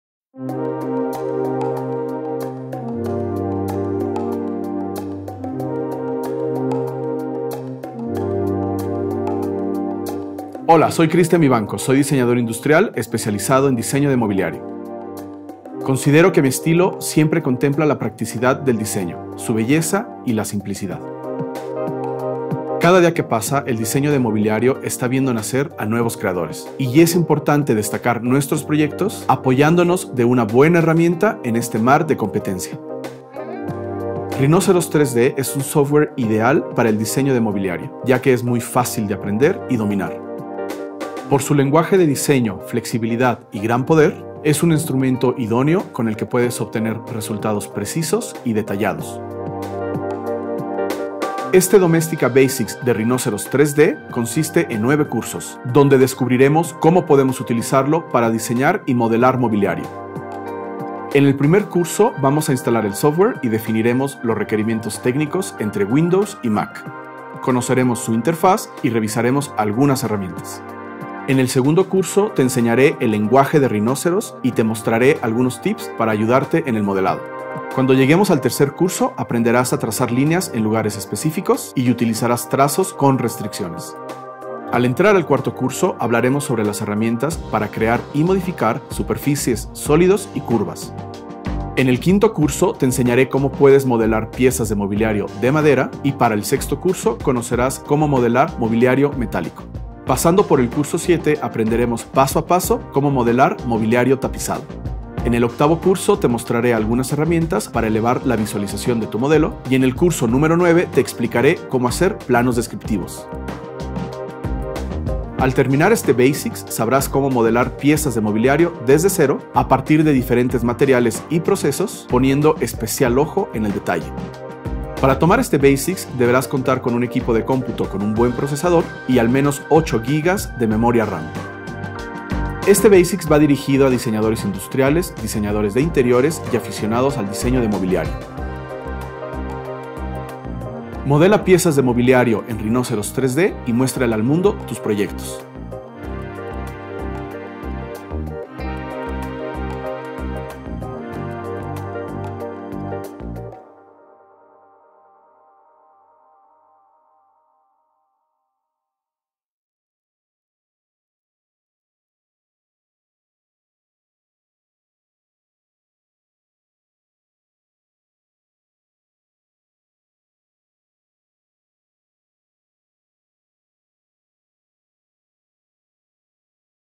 Introducción a Rhinoceros 3D para mobiliario - Curso Online de Christian Vivanco | Domestika
Aprende a usar las herramientas de Rhinoceros 3D desde cero, así como los procesos de trabajo para diseñar, modelar, visualizar y perfeccionar tu mobiliario en 3D

Junto a Christian Vivanco, diseñador industrial, aprenderás a modelar, visualizar y perfeccionar el diseño de muebles. Al finalizar este Basics de 9 cursos tendrás los conocimientos fundamentales para crear desde cero distintas tipologías de mobiliario, desde su dibujo inicial hasta un nivel de construcción básico.